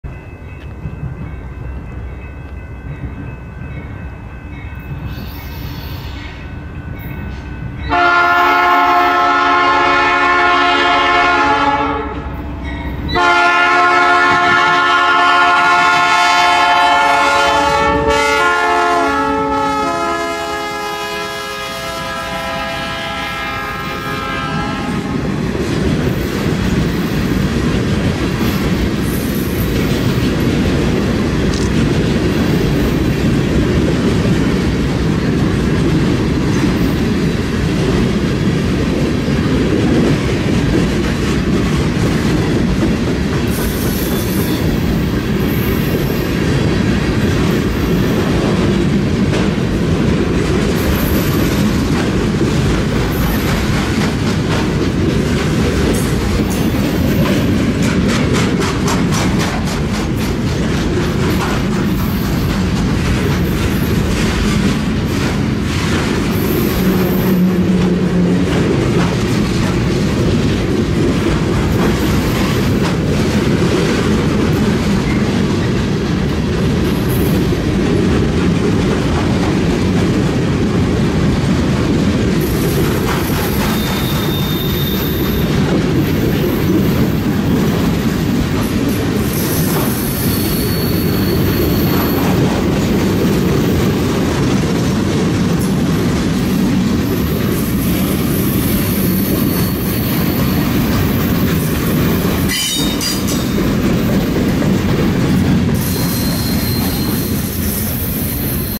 BNSF 971 Leads Military Train, Colona, IL 11/9/23
A BNSF H1 pulls military tanks on the Barstow sub. Not sure of the final destination. Symbol is U-CMYDEF.